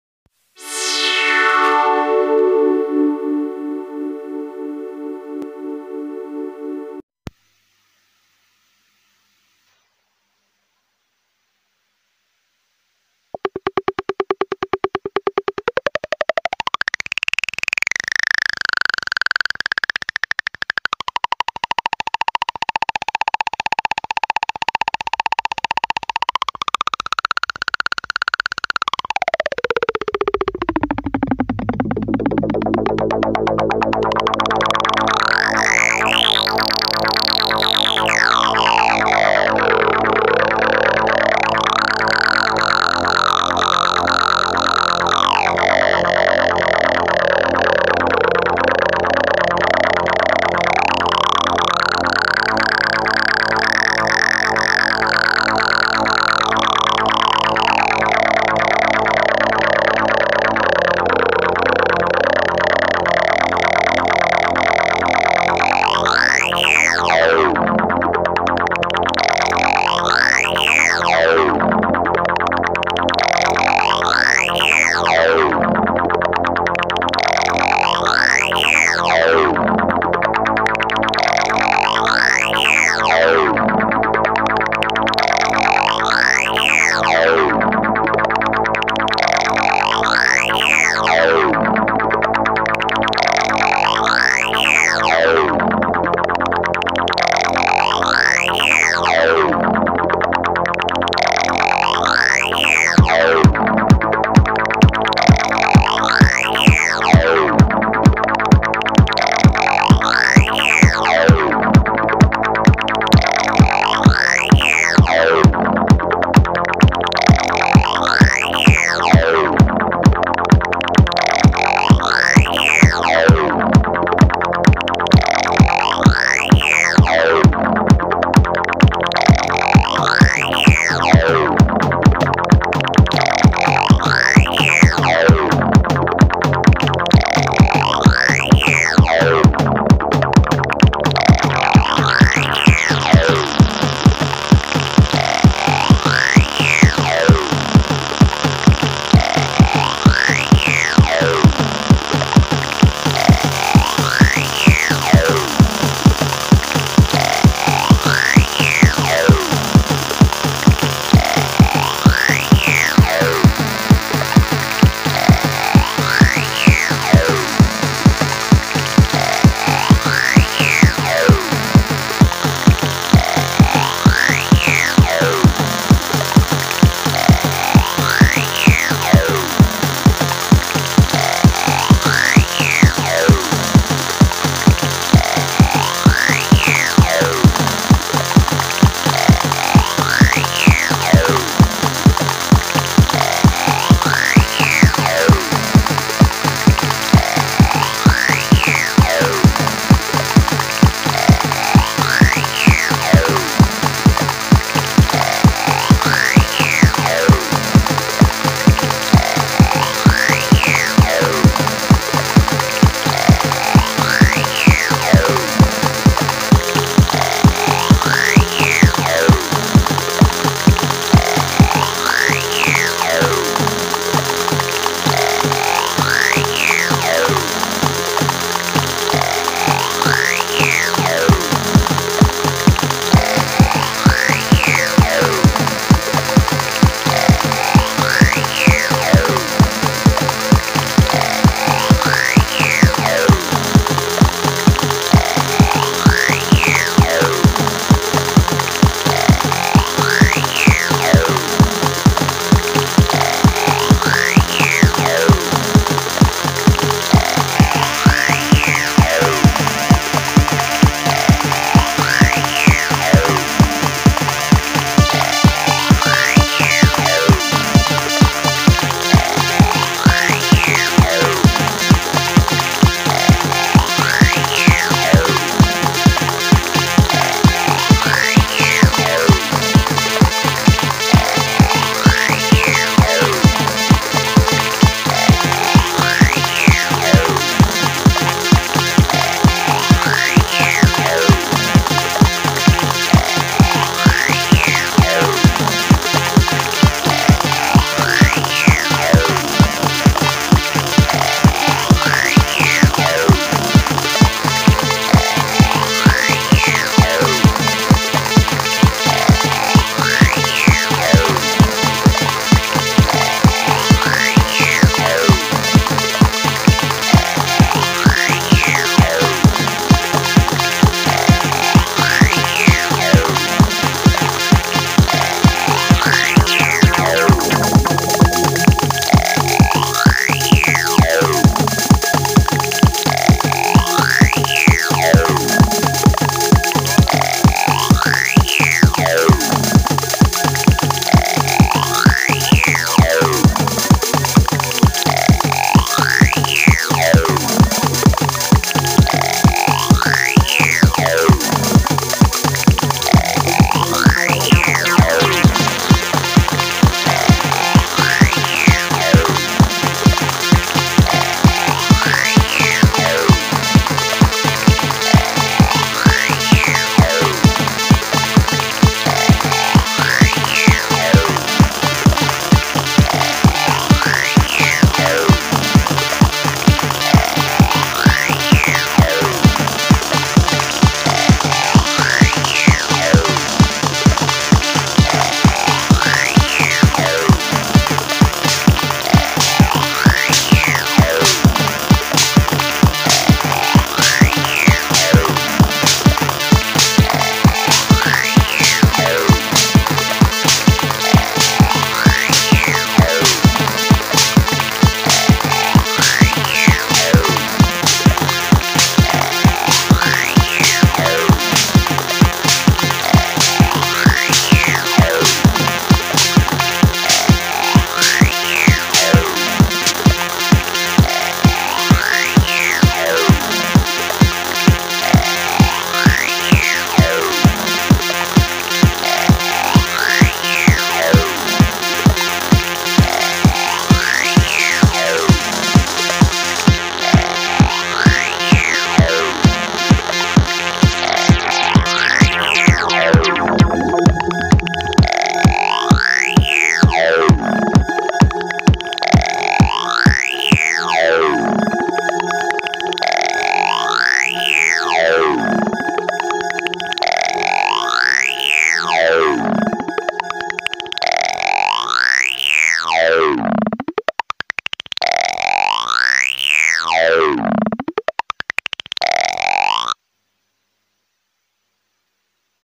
Techno with TR8, JU06A, Mininova, Kaossilator
Live Techno Synthesizer Performance
DAW-less setup
***used gear: ***

Drums : Roland TR8 , 909 Kit , Midi Master Clock
Arpeggio : Roland Ju06A
Pad Sound : Novation MiniNova
FX sound : Korg Kaossilator
Audio Interface : Alesis Core 1
Camera: Xiaomi Redmi Note 8 pro